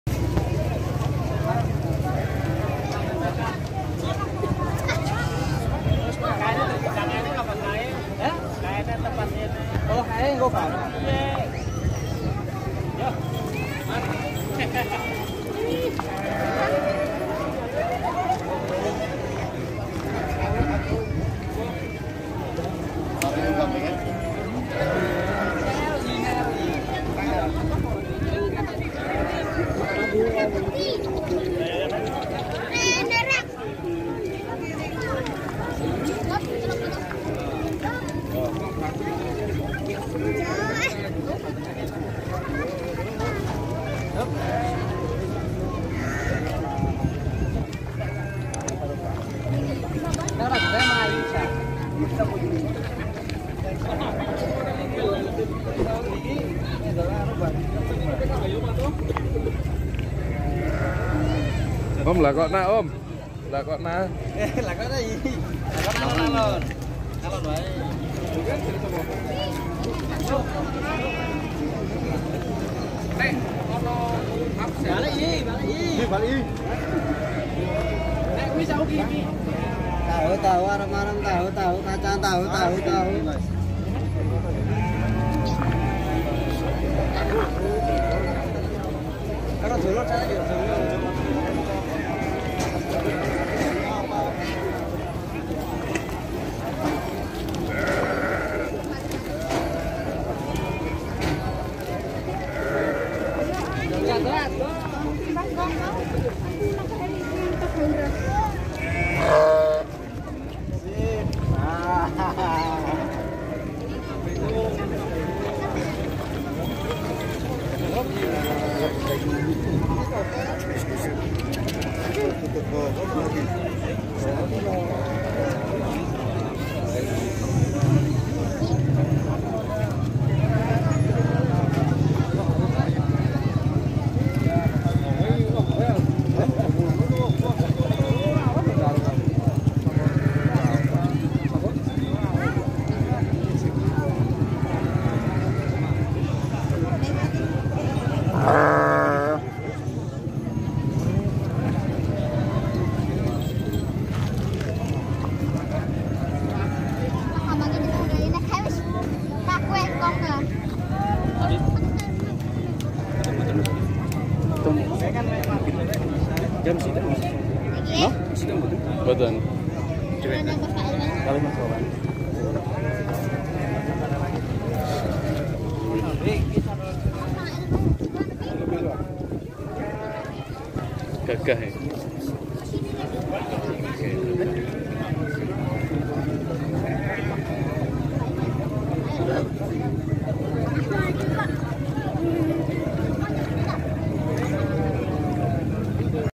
BOCIL NAIK DOMBOS/TEXEL WONOSOBO DI KONTES DOMBA WONOSOBO PIALA BUPATI 2022
BOCIL NAIK DOMBOS/TEXEL WONOSOBO DI KONTES BUPATI CUP 2022

INFORMASI MUDAL HIJAU MULTIFARM
Jalan Lurah Sudarto,Dusun Limbangan, Desa Mudal, Kec.Mojotengah, Kab.Wonosobo.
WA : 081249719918
FB  : Mudal Hijau Multifarm Wsb